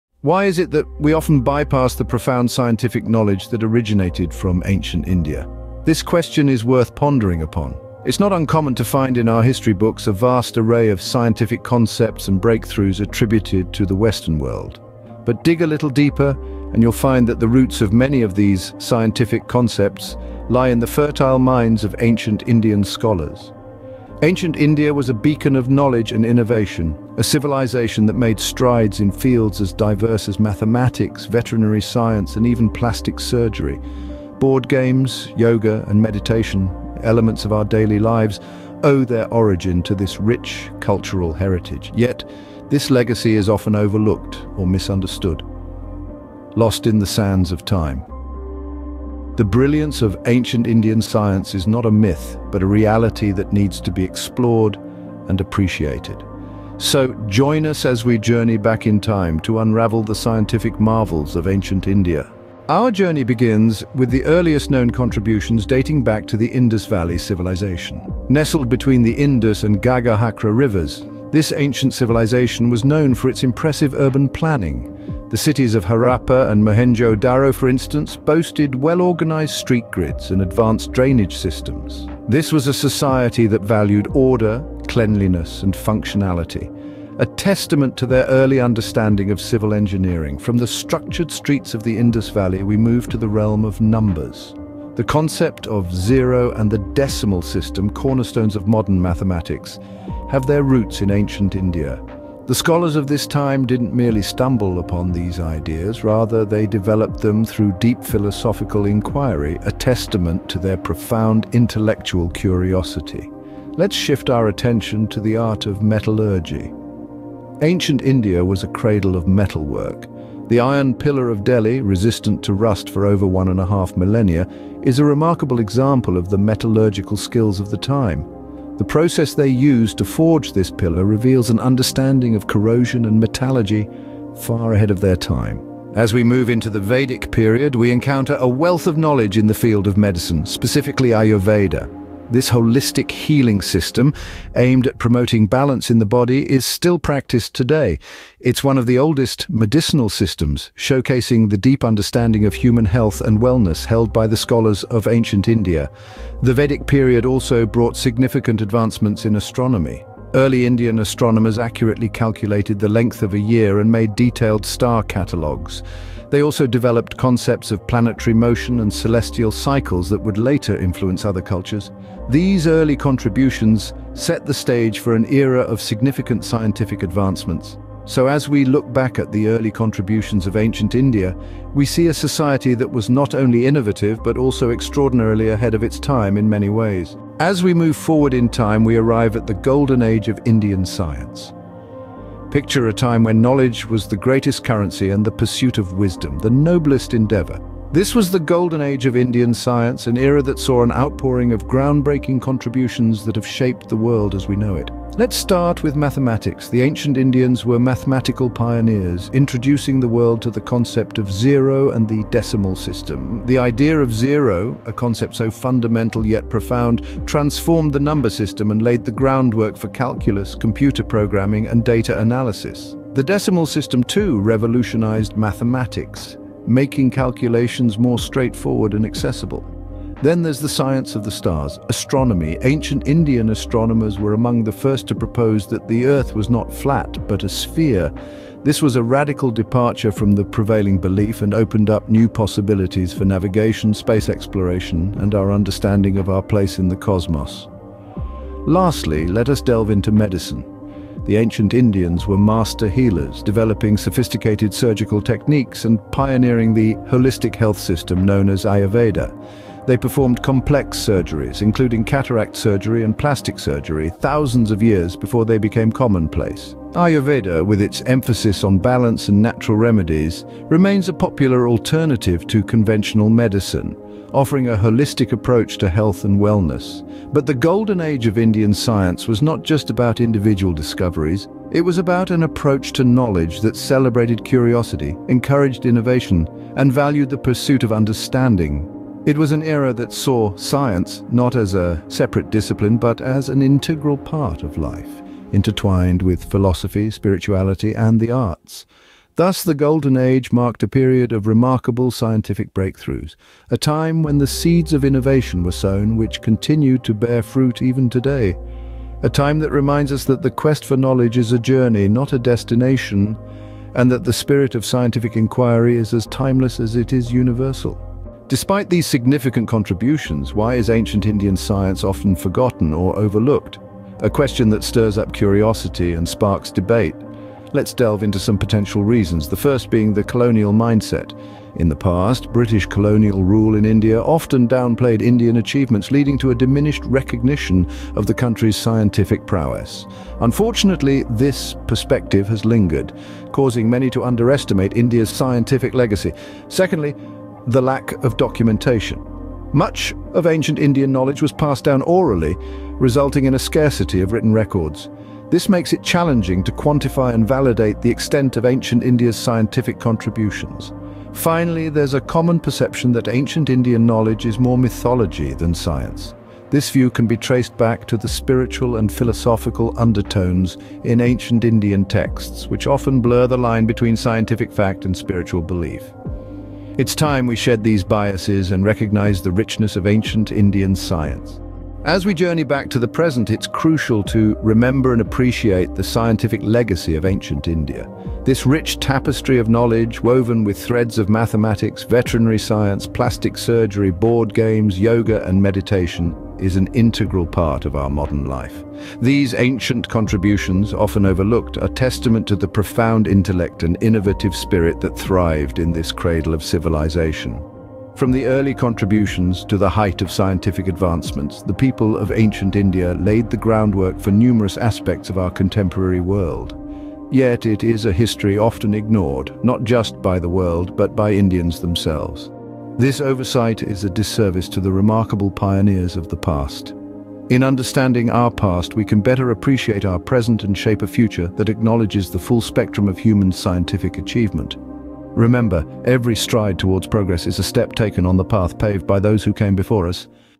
Ignored Legacy of Ancient Indian Science
Overlooked but immensely significant contributions of ancient Indian science. From advancements in mathematics and astronomy to remarkable achievements in medicine and technology, the rich scientific heritage of ancient India deserves more recognition. Join us as we uncover the reasons behind the underappreciation of Indian scientific achievements and shed light on the importance of acknowledging the past for a better future.

OUTLINE:
00:00:00 : The Overlooked Legacy of Ancient Indian Science

00:01:02 : The Early Contributions

00:03:07 : The Height of Scientific Advancements

00:05:18 : Why Ancient Indian Science is Overlooked

00:06:21 : Summary and Conclusion